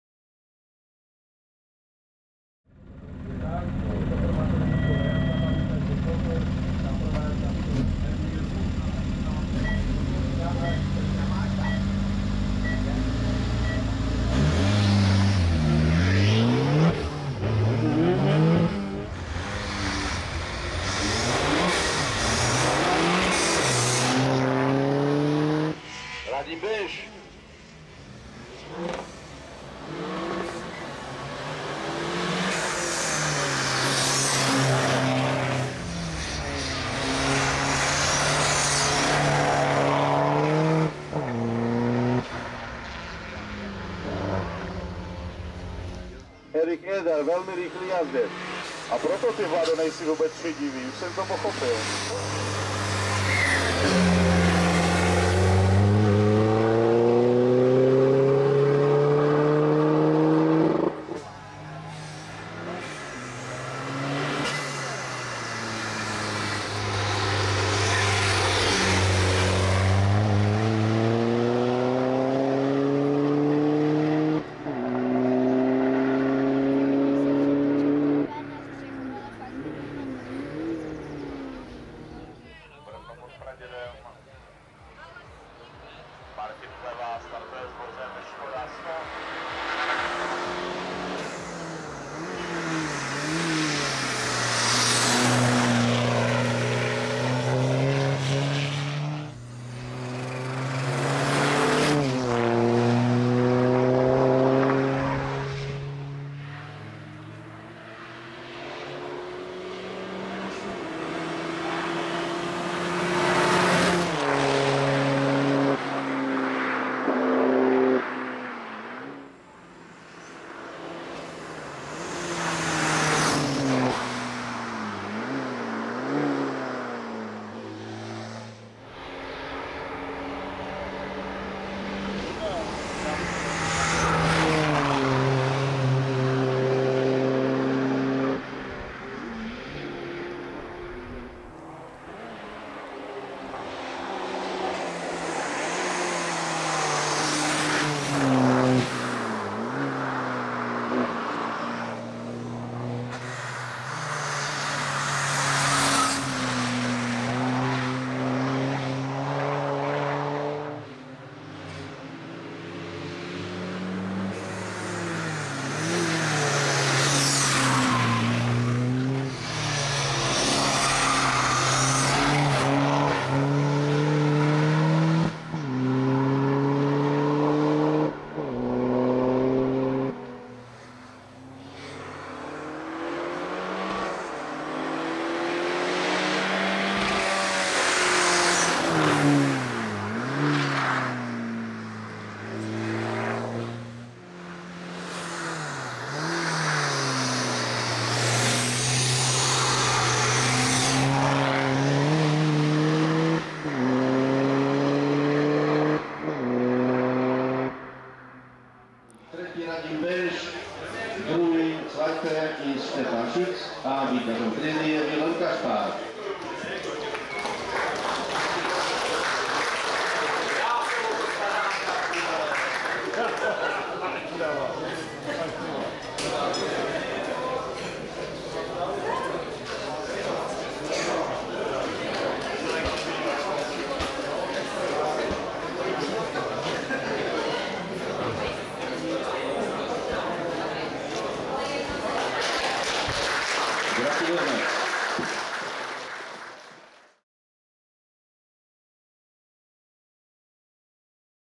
Škoda Fabia RS - Vilém Kašpar - IV. GMS Albrechtický kopec 2023 - Albrechtice u Lanškrouna
Vilém Kašpar si s vozem Škoda Fabia RS dojel po oba dva dny pro vítězství ve třídě S1-2000 v závodech do vrchu IV. GMS Albrechtický kopec 2023 "III. Memoriál Jiřího Lose".